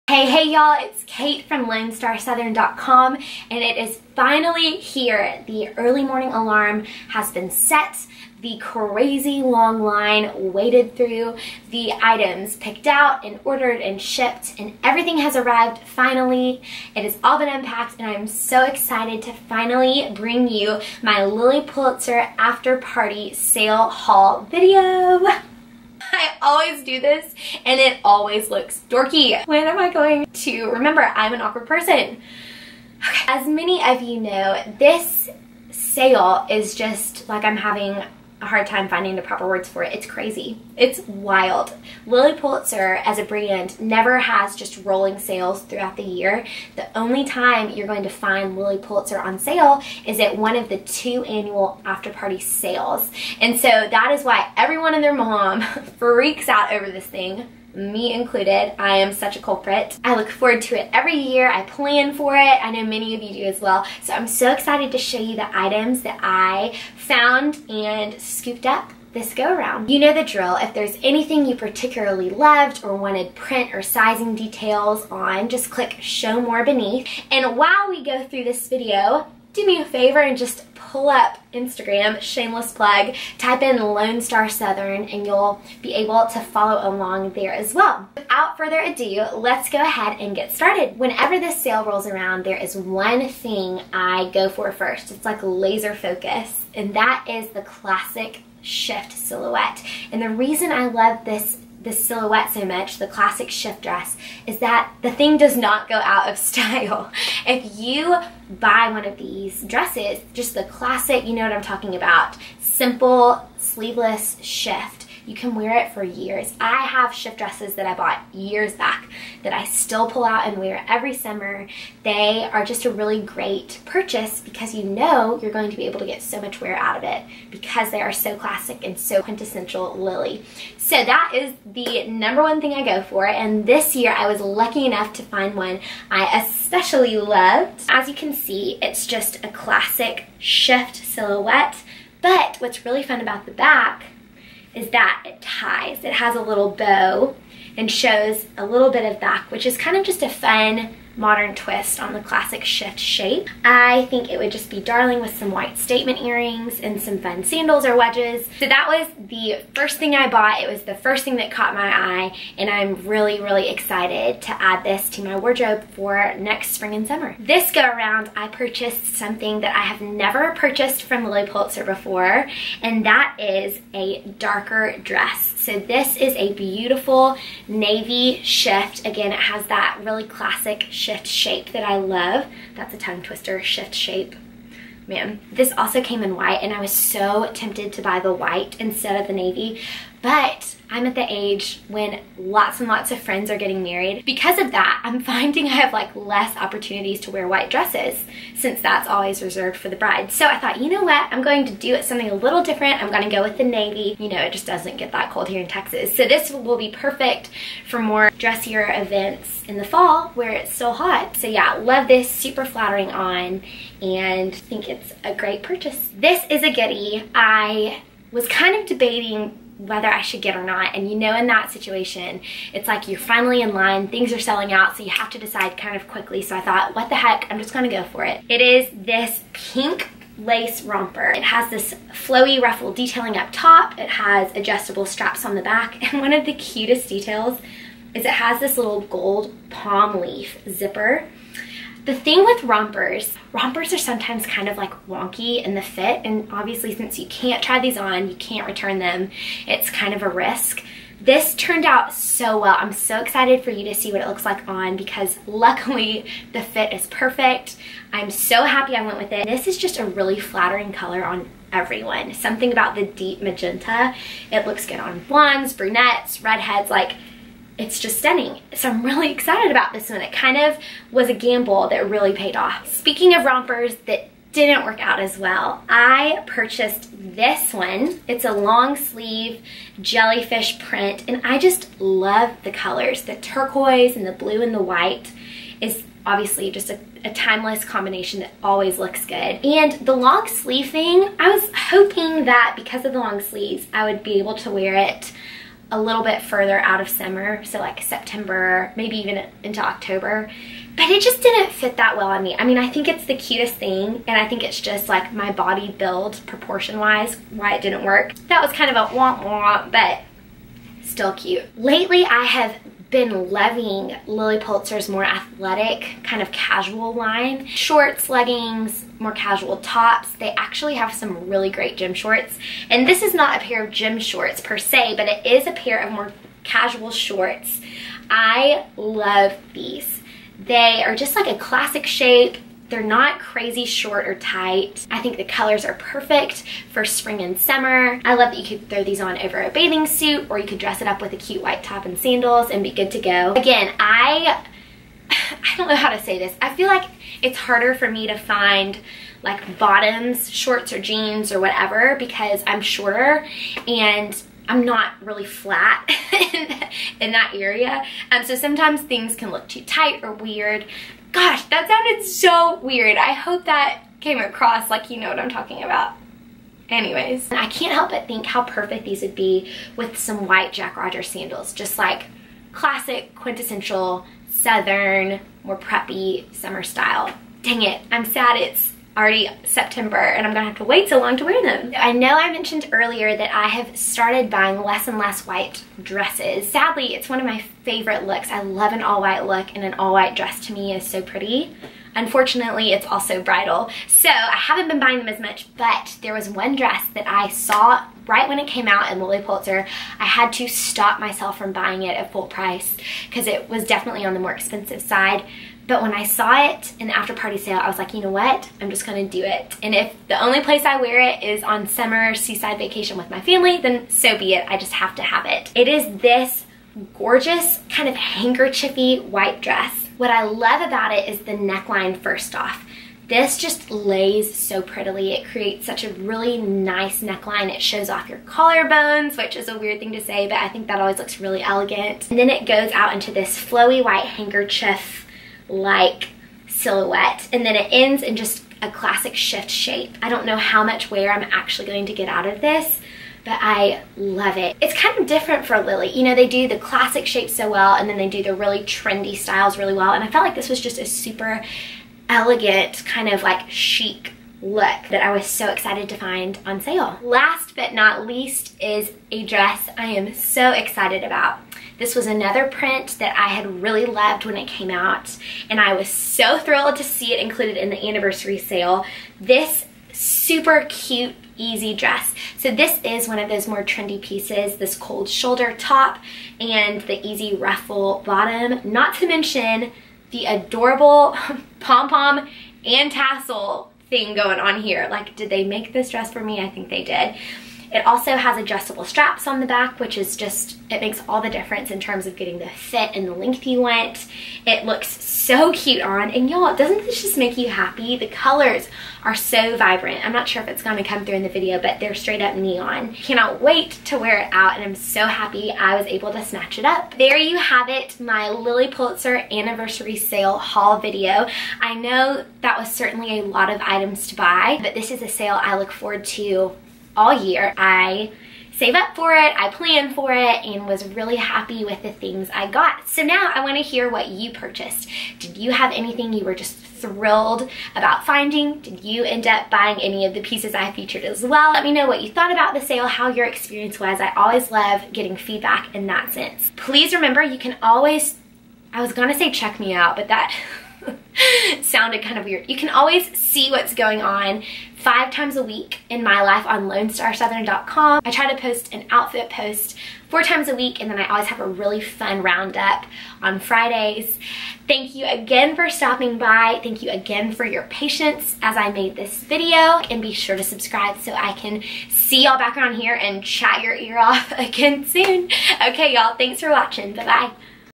LILLY PULITZER After Party Sale // August 2017
After a few shipping delays, everything has arrived and I'm SO excited to show y'all what I purchased in the Lilly Pulitzer After Party Sale! Let me know what you bought, and click "show more" for details: